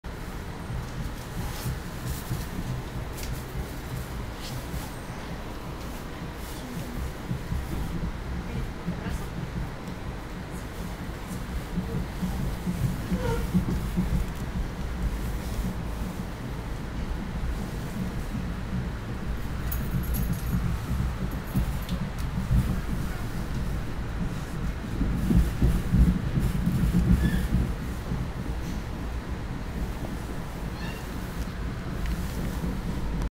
Leaving Red Bank, NJ aboard NJT Comet IIM 2/9/10
Leaving Red Bank, NJ aboard NJT Comet IIM going to Belmar, NJ (changing at Long Branch, NJ).